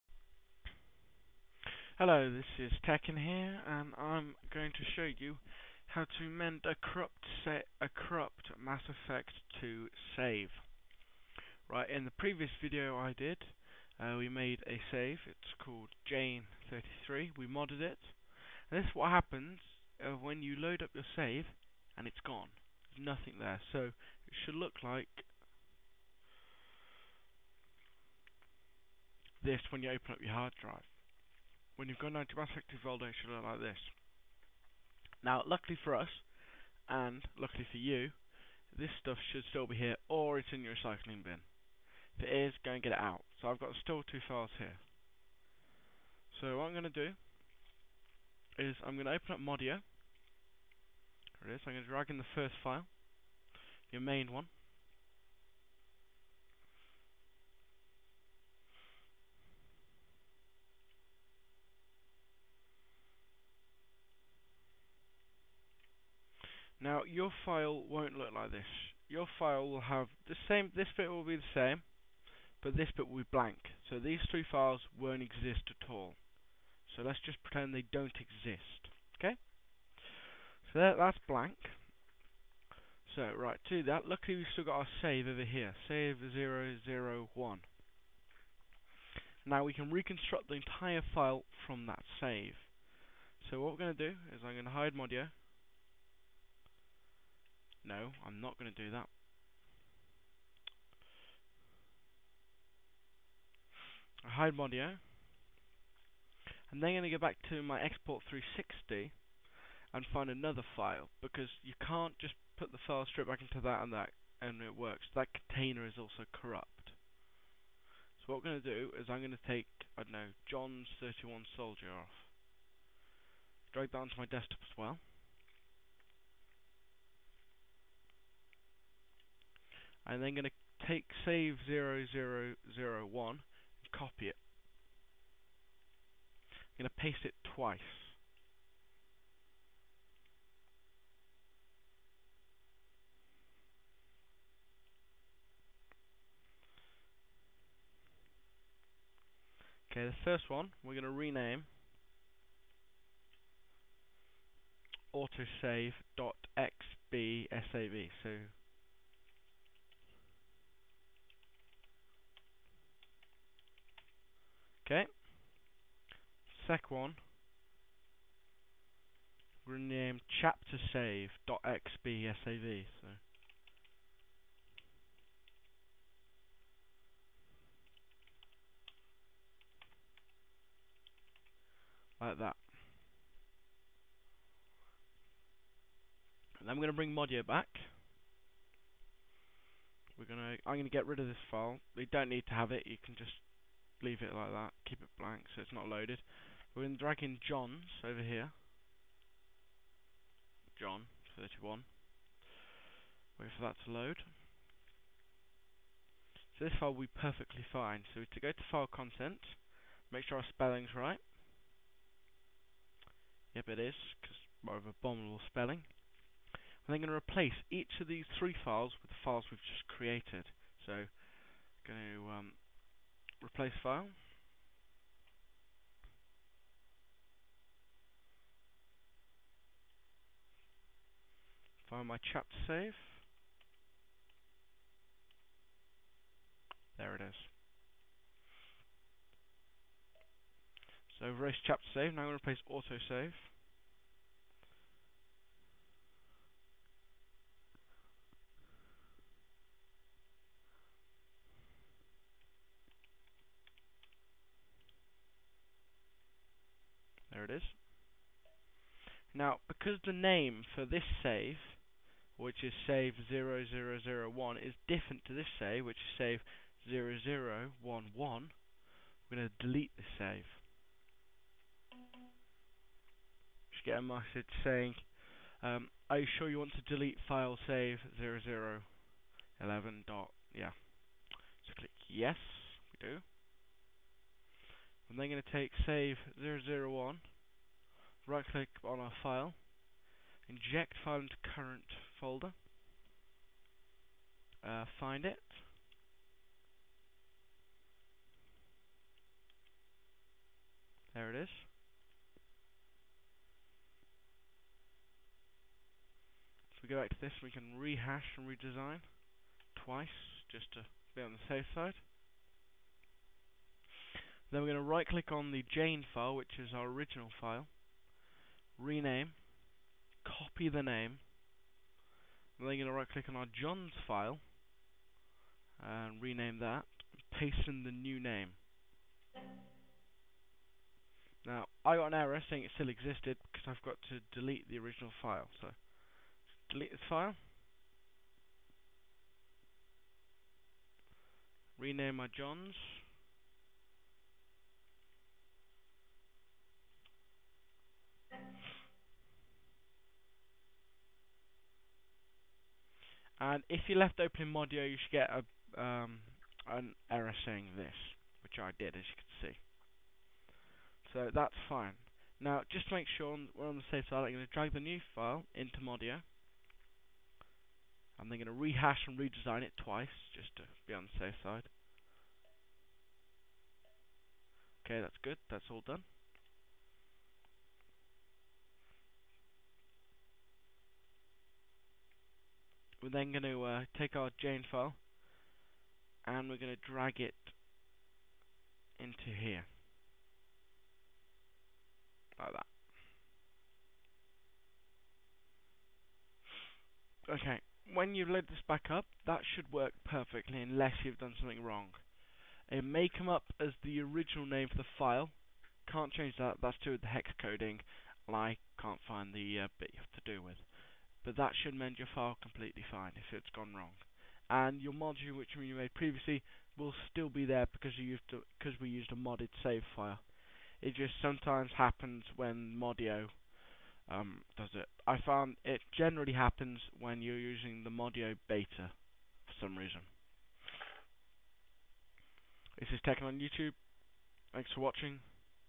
How to Repair a corrupt or missing Mass Effect 2 save game file - Voice Tutorial
This only works if you have modded the game save and still have the save files .